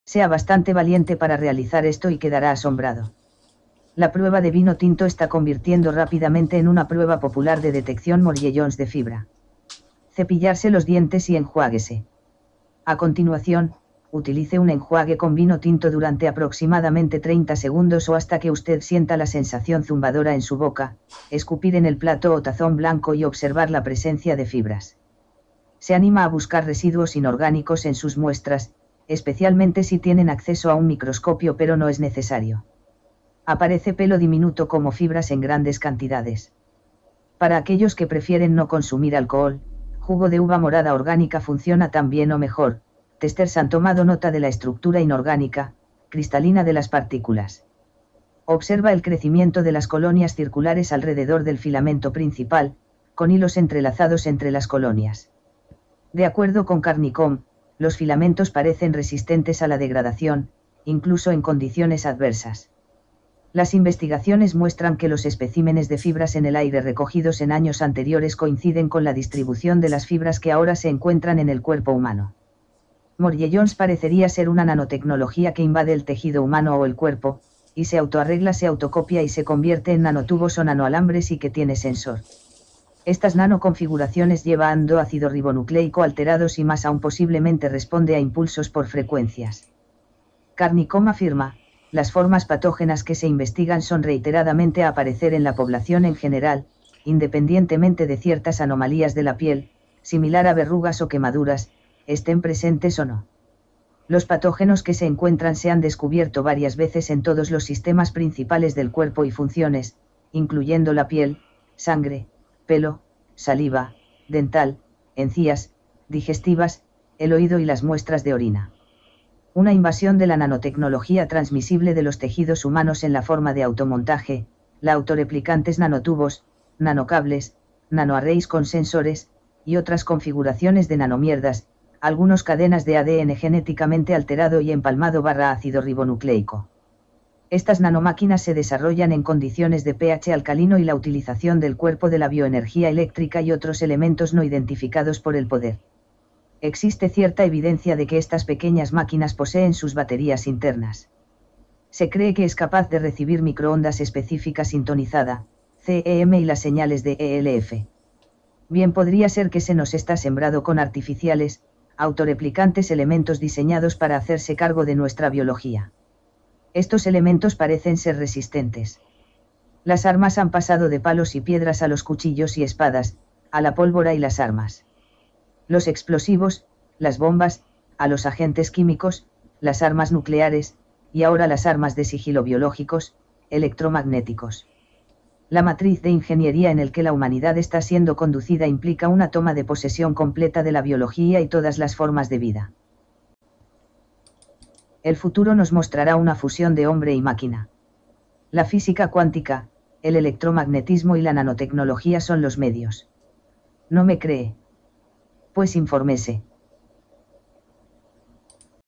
Morgellons - Test Del Vino Tinto - La Guerra Silenciosa
Sea bastante valiente para realizar esto y quedará asombrado.
La prueba de vino tinto está convirtiendo rápidamente en una prueba popular de detección Morgellons de fibra. Cepillarse los dientes y enjuagué su boca con agua. A continuación, utilice un enjuague con vino tinto durante aproximadamente 30 segundos o hasta que usted sienta la sensación zumbadora en su boca, Escupir en el  plato o tazón blanco y observar la presencia de fibras. Se anima a buscar residuos inorgánicos en sus muestras, especialmente si tienen acceso a un microscopio pero no es necesario.  Aparece Pelo diminuto como fibras en grandes cantidades.
(Para aquellos que prefieren no consumir alcohol, jugo de uva morada orgánica funciona tan bien o mejor) Testers han tomado nota de la estructura inorgánica, cristalina de las partículas.

Observa el crecimiento de las "colonias" circulares alrededor del filamento principal, con hilos entrelazados entre las colonias. De acuerdo con Carnicom, los filamentos parecen resistentes a la degradación, incluso en condiciones adversas. Las investigaciones muestran que los especímenes de fibras en el aire recogidos en años anteriores coinciden con la distribución de las fibras que ahora se encuentran en el cuerpo humano.

MORGELLONS parecería ser una nano-tecnología que invade el tejido humano o el cuerpo, y se auto-arregla se auto-copia y se convierte en nano-tubos o nano-alambres y que tiene sensor. estas nano configuraciones lleva AND o ARN alterados y mas aun posiblemente responde a impulsos por frecuencias.

Carnicom afirma: "Las formas patógenas que se investigan son reiteradamente a aparecer en la población en general, independientemente de ciertas "anomalías de la piel" (similar a verrugas o quemaduras) estén presentes o no. los patógenos que se encuentran se han descubierto varias veces en todos los sistemas principales del cuerpo y funciones, incluyendo la piel, sangre, pelo, saliva, dental (encías), digestivas, el oído y las muestras de orina.

Una invasión de la nanotecnología transmisible de los tejidos humanos en la forma de auto-montaje, la auto-replicantes nanotubos, nano-cables, nanoarrays con sensores, y otras configuraciones de "nanomierdas", algunas cadenas de ADN genéticamente alterado y empalmado / ARN. Estas nanomáquinas se desarrollan en condiciones de pH alcalino y la utilización del cuerpo de la bio-energía eléctrica y otros elementos no identificados por el poder. Existe cierta evidencia de que estas pequeñas máquinas poseen sus baterías internas. Se cree que es capaz de recibir microondas específica sintonizada, CEM y las señales de ELF.

Bien podría ser que se nos está sembrado con artificiales, auto-replicantes elementos diseñados para hacerse cargo de nuestra biología. Estos elementos parecen ser resistentes.

Las armas han pasado de palos y piedras a los cuchillos y espadas, a la pólvora y las armas,los explosivos, las bombas, a los agentes químicos, las armas nucleares, y ahora las armas de sigilo-biológicos, electromagnéticos. La matriz de ingeniería en el que la humanidad está siendo conducida implica una toma de posesión completa de la biología y todas las formas de vida. El futuro nos mostrará una fusión de hombre y máquina. La física cuántica, el electromagnetismo y la nanotecnología son los medios. ¿No me cree? PUES INFORMESE.

Información extraída del Usuario: MrMaxBliss
Su vídeo: Chemtrails/Morgellons : Red Wine Test

PD: el vino que utilice es Campos de Oro, que lo hace un amiguete mio y esta de lujo, completamente biologico, yo no soy bebedor pero me daba una pena tener que sacarlo de la boca :(